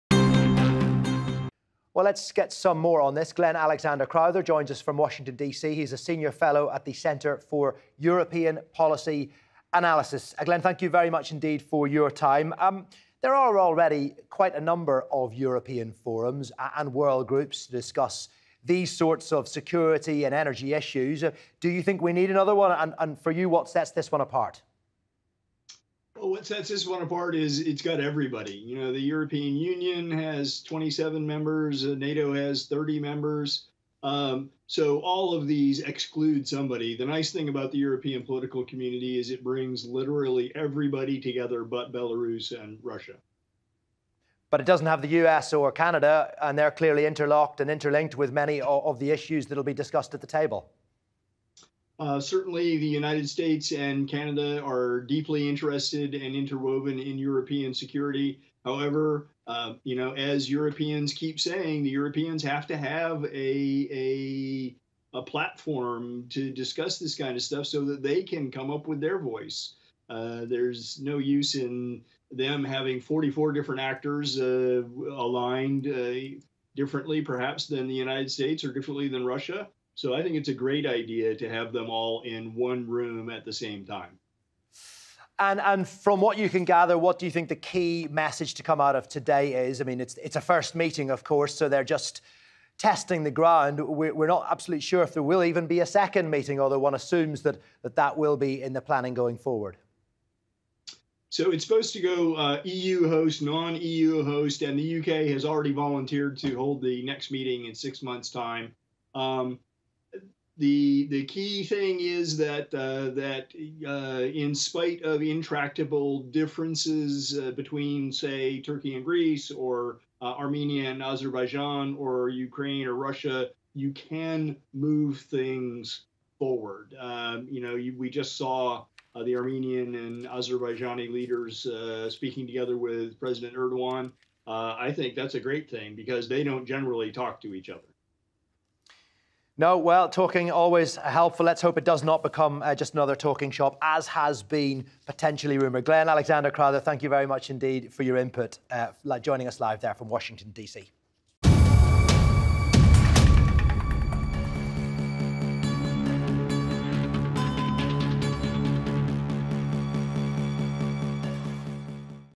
What’s the significance of the new European Political Community?
The leaders of 44 countries from across the European continent have been holding a series of talks in Prague. It’s the first meeting of the newly formed European Political Community. The idea of a gathering with a broader membership had been championed by French President Emmanuel Macron. Alexander Crowther from the Center for European Policy Analysis explains how this forum can set itself apart from others.